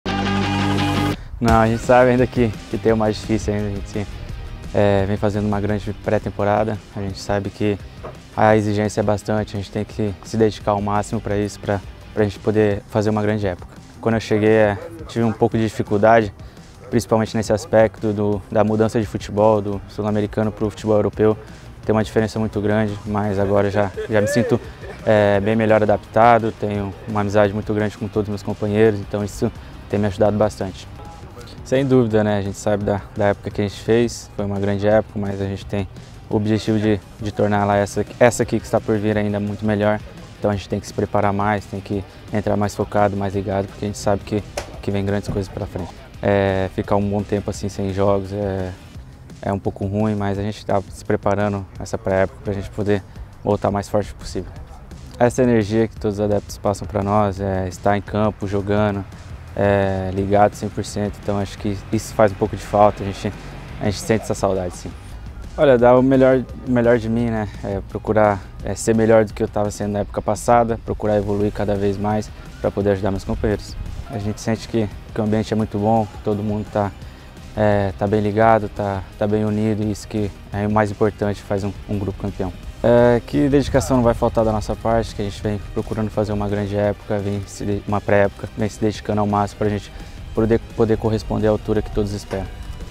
Declarações Pepê 🎙O estágio, a pré-temporada e muito mais
🎙 "A exigência da pré-temporada é enorme para fazermos uma grande época"

Pepê abordou o estágio e a pré-temporada da nossa equipa 💪